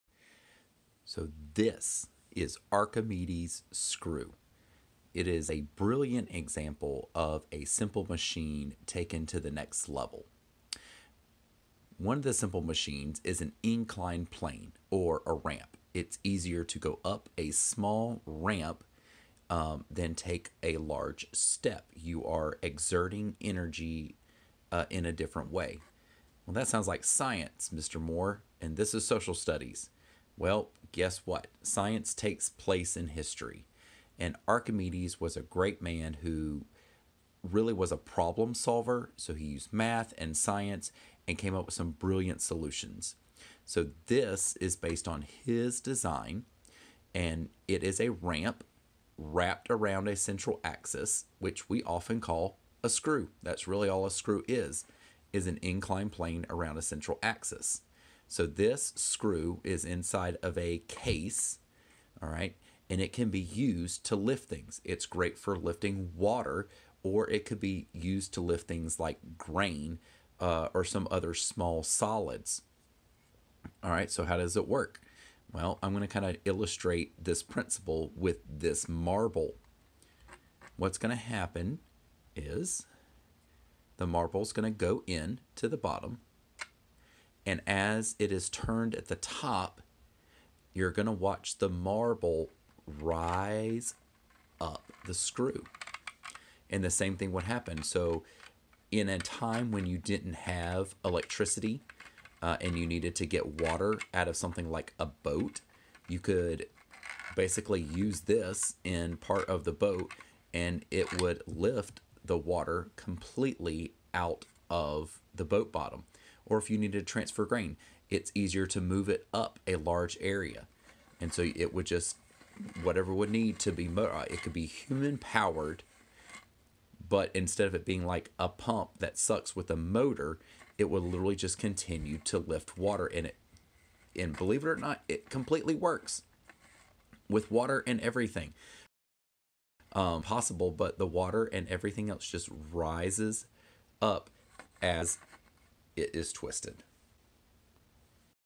Archimedes' Screw demonstration
Using a 3D model of Archimedes' Screw to demonstrate his ingenious invention